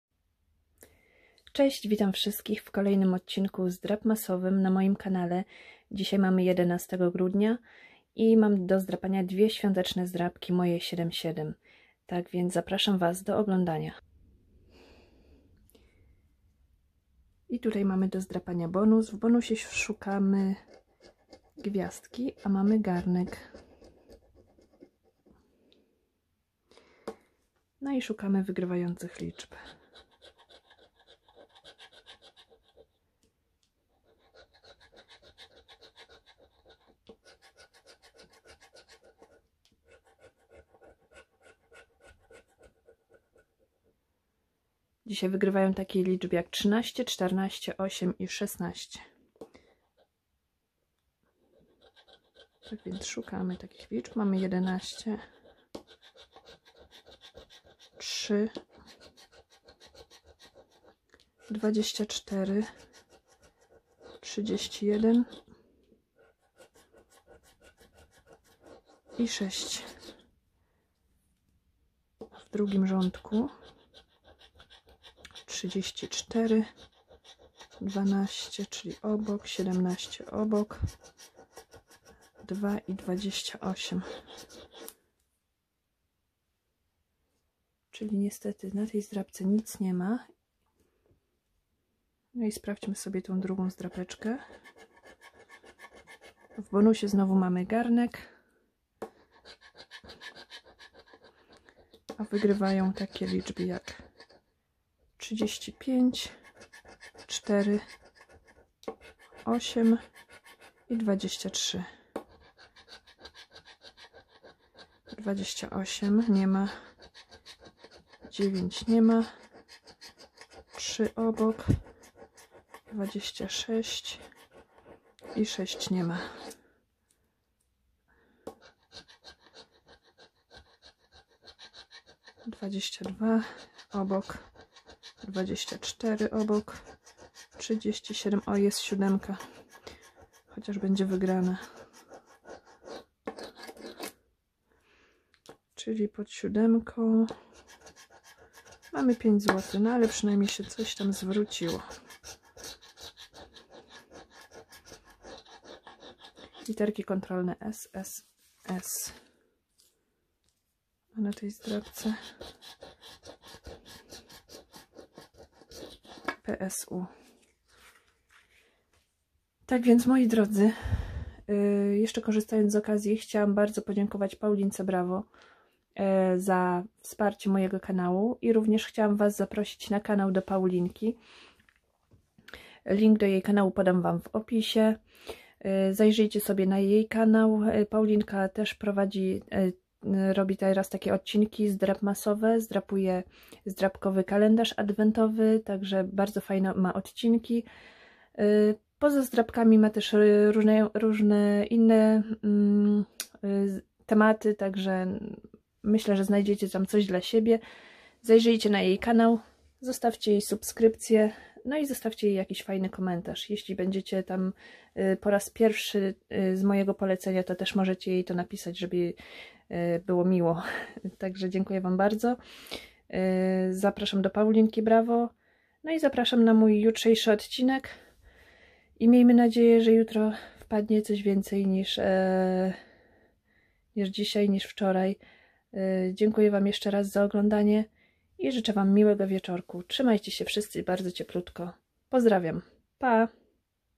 #75 Zdrapki🎄SCRATCHMAS #11🎄 Moje 77❄Paulinka Bravo😀📢
Witam w 11 dniu wyzwania zdrapkowego Zdrapmas. Odcinki codziennie os 1 do 24 grudnia. Zapraszam i życzę miłego oglądania 😊👋